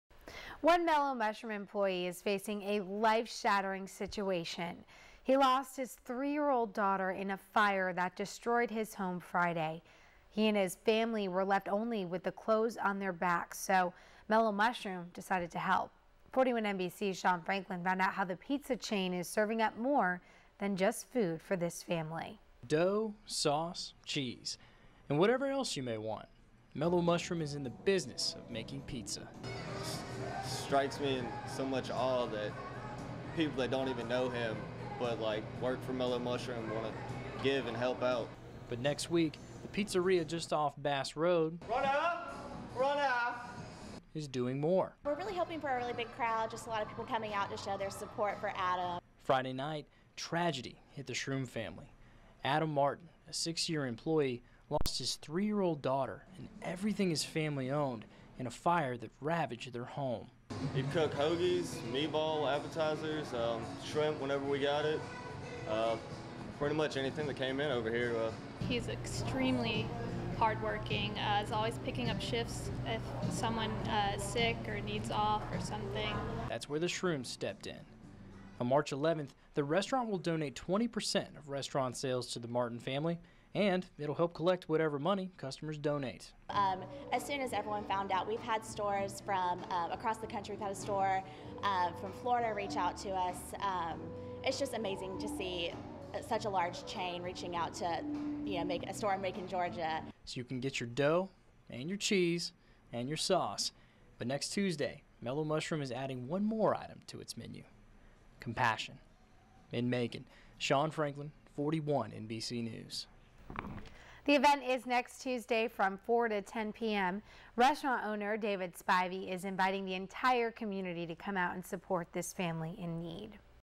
41NBC/WMGT - Mellow Mushroom Helps an Employee in Need - 3.04.14
A  local Mellow Mushroom employee is facing a life shattering situation so Mellow Mushroom decided to help.